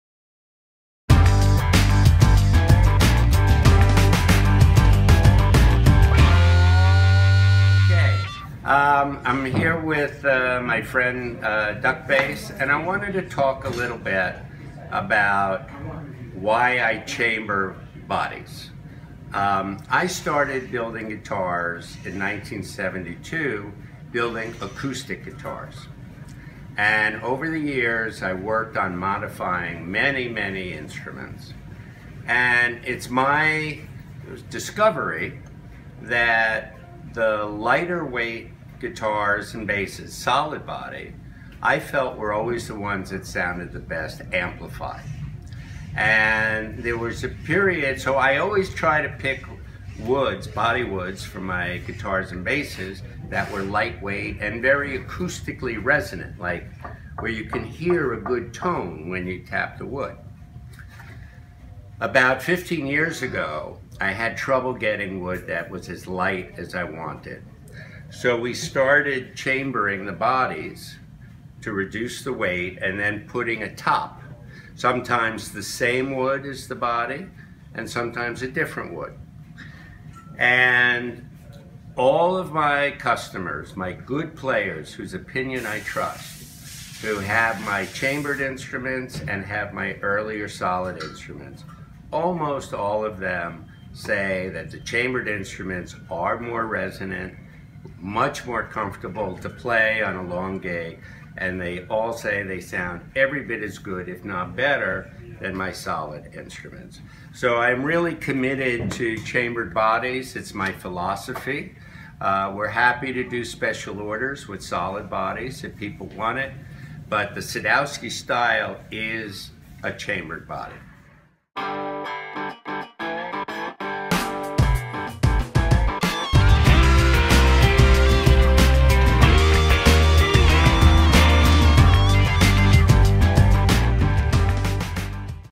Sadowsky Chambered Style
새도우스키 챔버 바디에 대한 로저 새도우스키 인터뷰